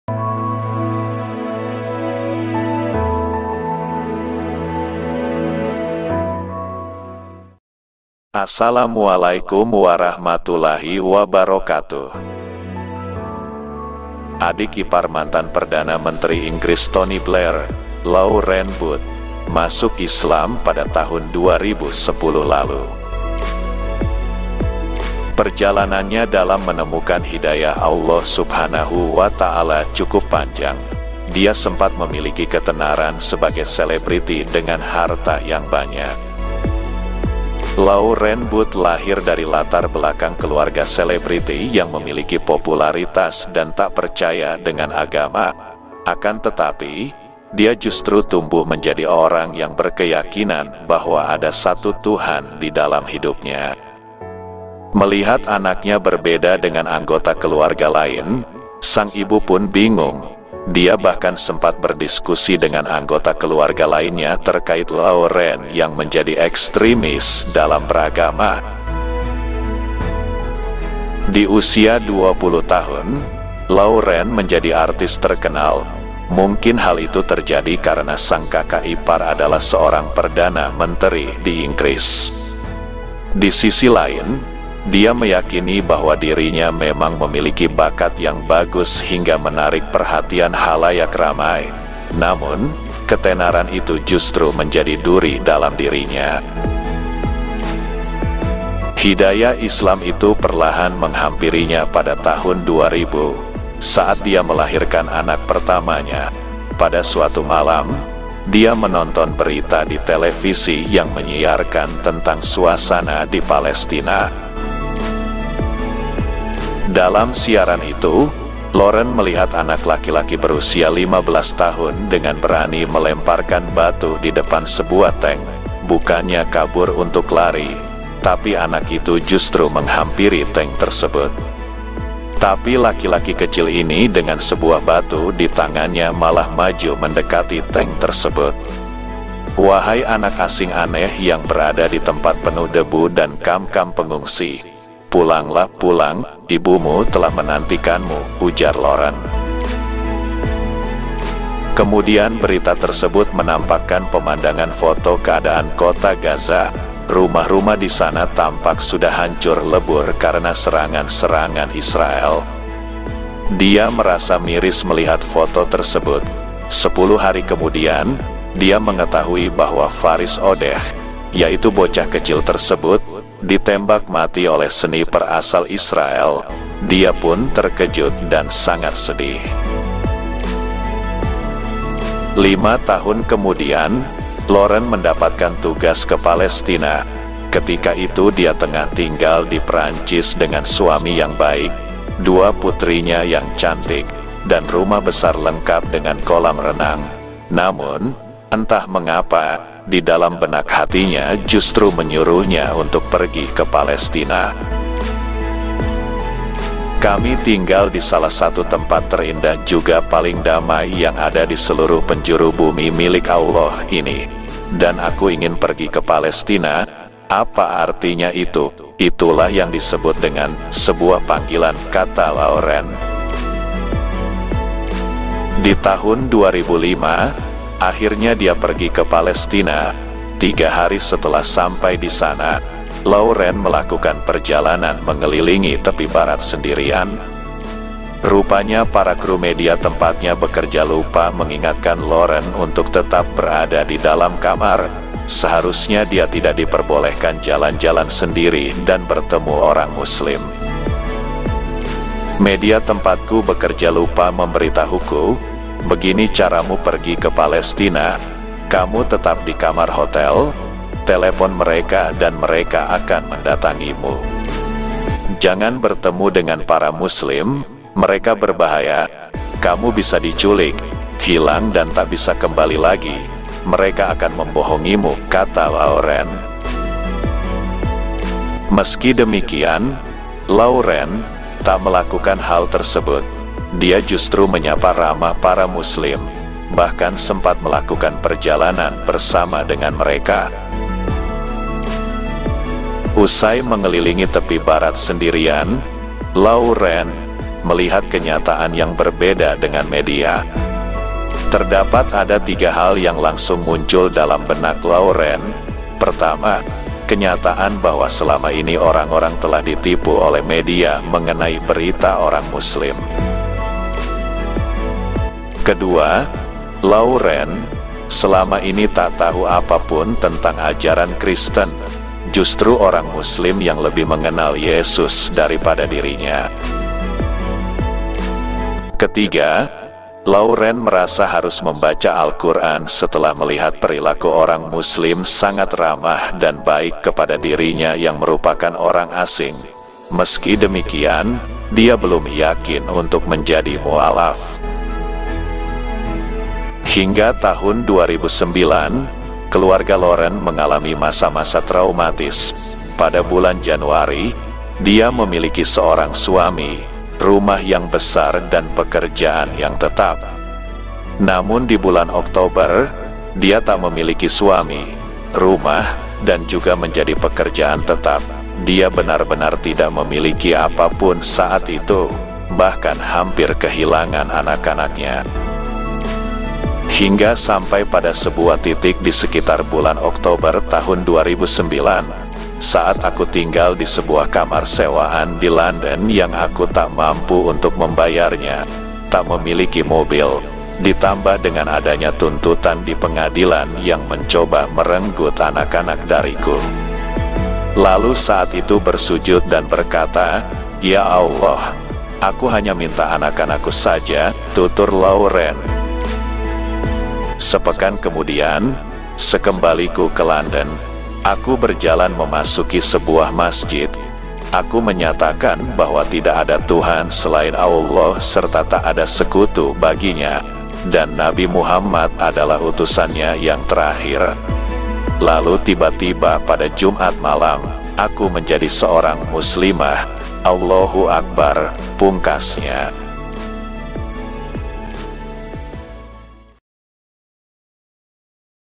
Kisah Mualaf || Selebritis Laurent Booth Ipar Mantan PM Inggris Masuk Islam
lauren Booth, adik ipar mantan perdana menteri Inggris Tony Blair, menjadi mualaf pada tahun 2010. Perjuangannya dalam menemukan hidayah dari Allah Subhanahu wa ta'ala tergolong cukup panjang. Terlebih lagi dia sempat memiliki ketenaran sebagai selebriti dengan harta yang melimpah.

Lauren Booth terlahir dari latar belakang keluarga selebriti yang memiliki popularitas dan tak percaya dengan agama. Namun, dia justru tumbuh menjadi orang yang berkeyakinan bahwa ada satu Tuhan di dalam hidupnya. Melihat anaknya berbeda dengan anggota keluarga lain, sang ibu pun bingung. Dia bahkan sempat berdiskusi dengan anggota keluarga terkait sosok Lauren yang menjadi ekstrimis dalam beragama.

Menginjak usia 20 tahun, Lauren menjadi artis terkenal. Mungkin hal itu terjadi karena sang kakak ipar adalah seorang perdana menteri. Di sisi lain, dia yakin bahwa dirinya memang memiliki bakat yang mumpuni hingga menarik perhatian khalayak. Namun, ketenaran itu justru menjadi racun dalam dirinya.

Hidayah Islam itu perlahan muncul pada tahun 2000, ketika dia melahirkan anak pertamanya. Suatu malam, dia menonton berita di televisi yang menyiarkan tentang kabar Palestina. Dalam siaran itu, Lauren melihat anak laki-laki berusia 15 tahun dengan berani melemparkan batu di depan sebuah tank. Bukannya kabur untuk lari, anak itu justru menghampiri tank tersebut.

Jika tank itu mengarah pada kita, dalam situasi di mana kita berada sekarang. Kita akan lari ke arah berlawanan kan. Itu insting manusia. Tapi anak laki-laki kecil ini dengan sebuah batu di tangannya malah maju mendekati tank tersebut. Aku hanya berkata dalam hari: 'Larilah wahai bocah Timur Tengah yang aneh. Wahai anak asing aneh yang berada di tempat penuh debu dan kamp-kamp pengungsi. Pulanglah! Pulang! Ibumu telah menantikanmu'," ujar Lauren.